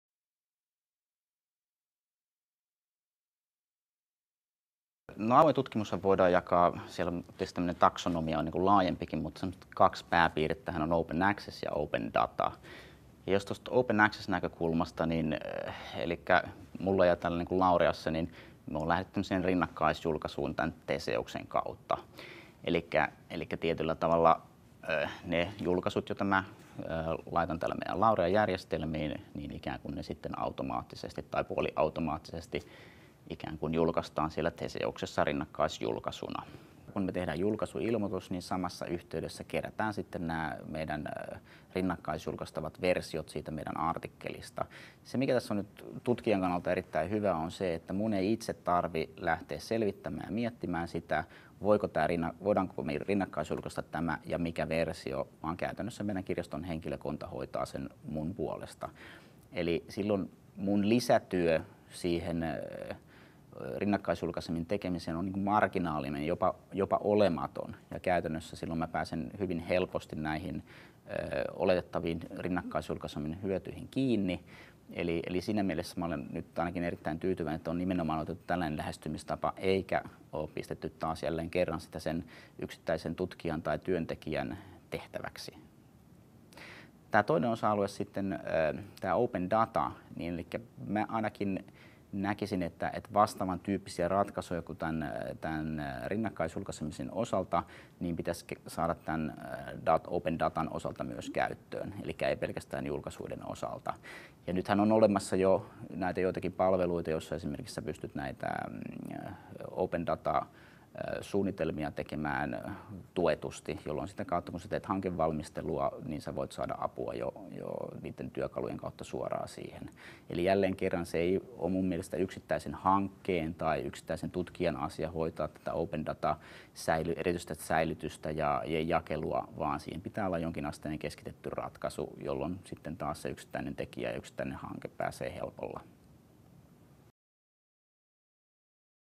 Teemu Santonen – Avoimuus ammattikorkeakoulujen TKI-toiminnassa
Teemu Santonen, yliopettaja, KTT, Laurea-ammattikorkeakoulu, kertoo avoimuudesta (open access, open data) ammattikorkeakoulujen TKI-toiminnassa.